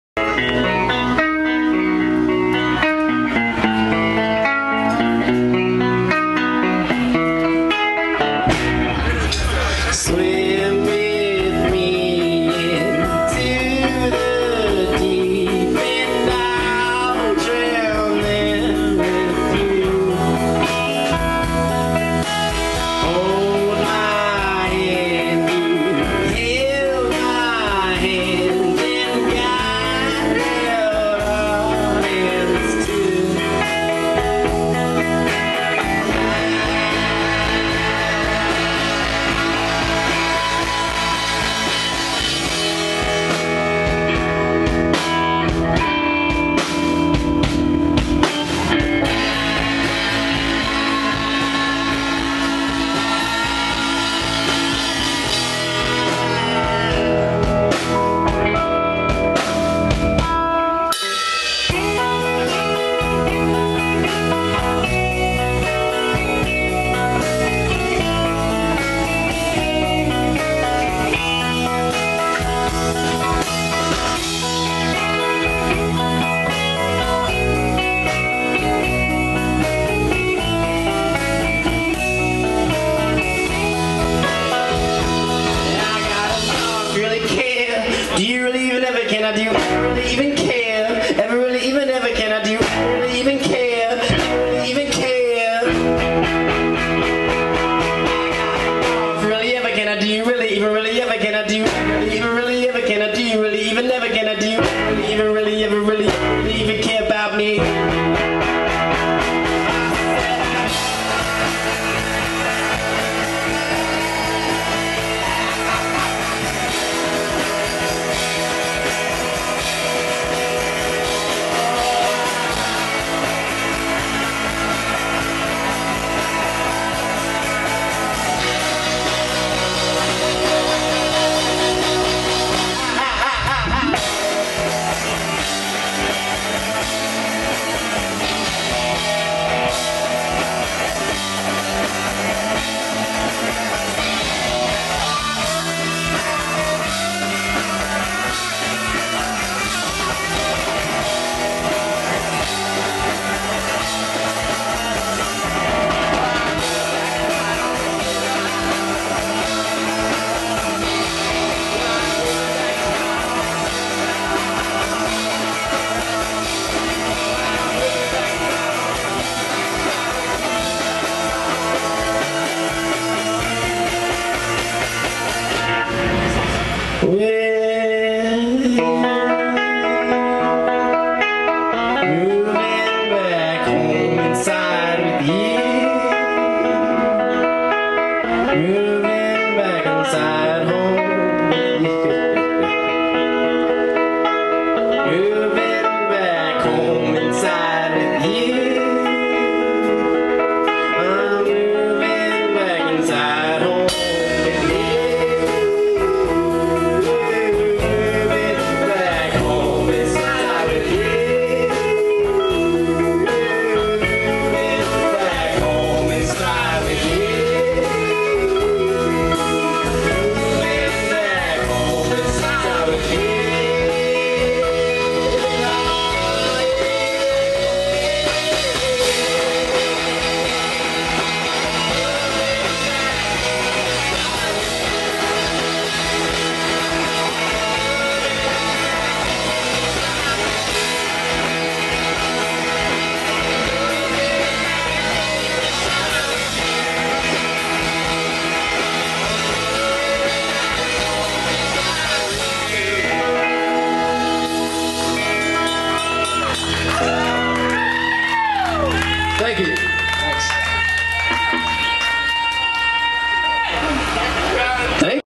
Lonnie Walker - Unknown Song (Raleigh Wide Open)
live @ the Cherry Bounce Stage at Raleigh Wide Open - 5 September 2008